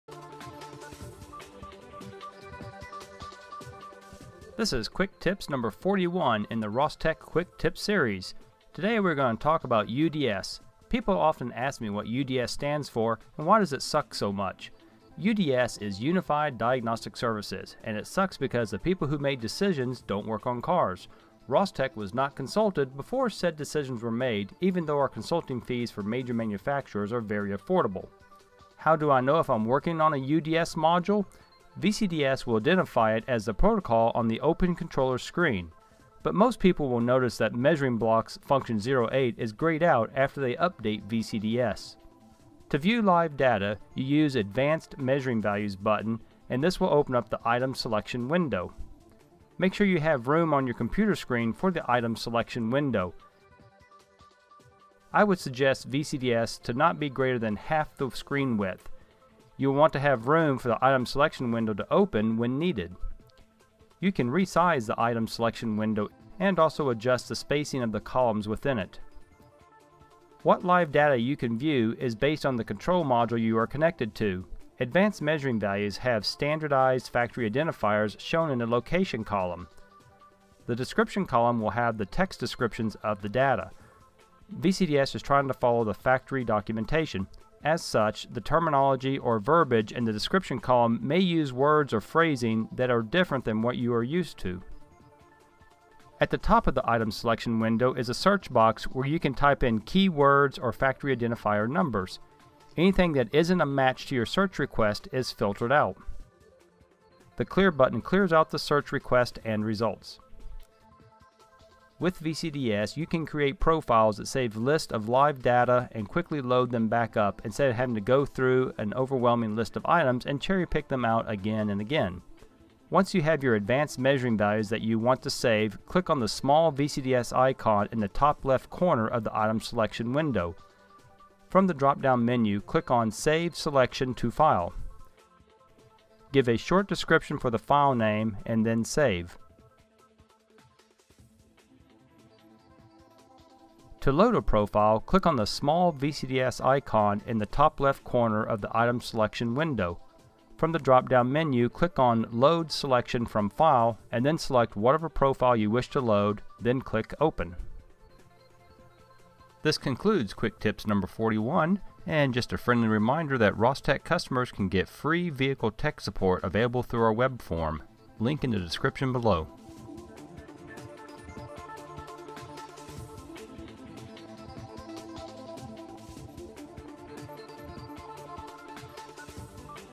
Quick Tips #41: UDS Measuring Values
QT#41, as always, some quick talking points about some aspect using VCDS, this time,  dealing with UDS modules.  Special shout out to our good friend Jack... I can't be a jerk to people who deserve it, but he can, so we all love Jack.